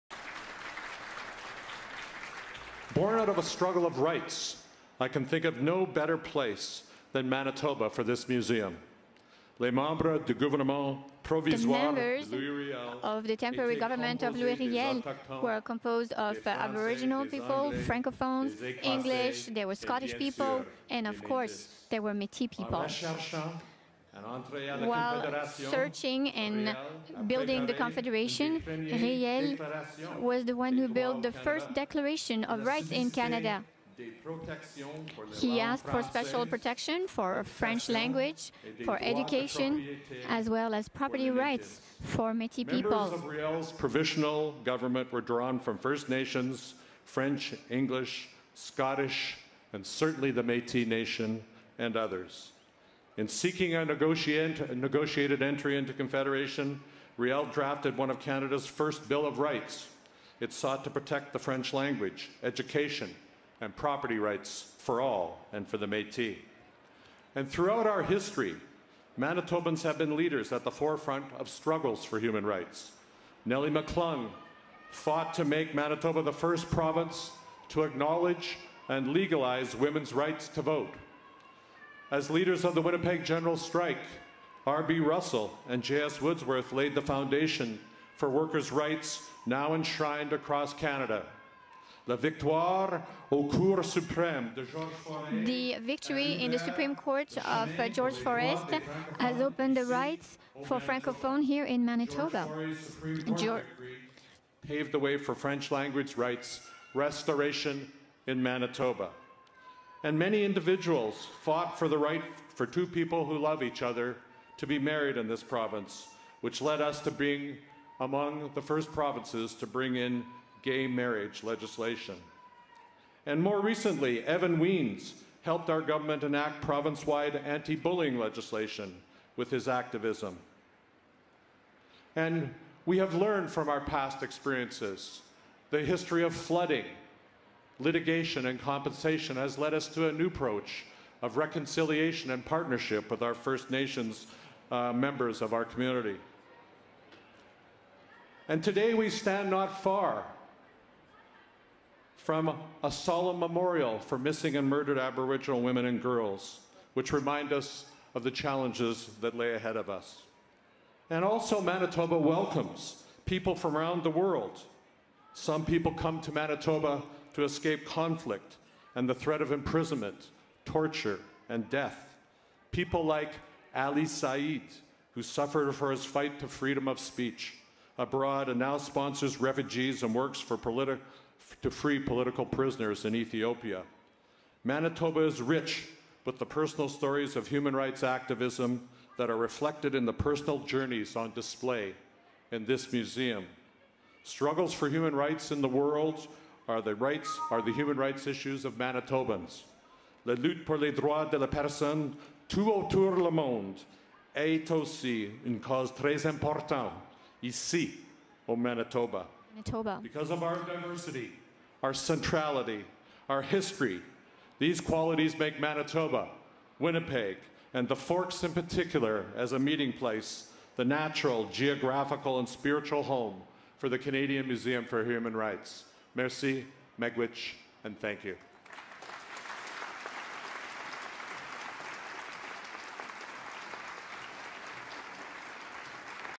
CMHR Opening Ceremonies - Premier Greg Selinger
Why is Manitoba the right home for the first national museum outside of Ottawa?

From historic figures like Nellie McClung, R.B. Russell, J.S. Woodworth, and Georges Forrest to new champions of equality like Evan Weins and Ali Saeed, Premier Greg Selinger reminds us that "Manitobans have always been leaders at the forefront of struggles for human rights".

Listen to his speech from the opening ceremonies of the Canadian Museum for Human Rights. Footage courtesy of the livecast provided by the museum.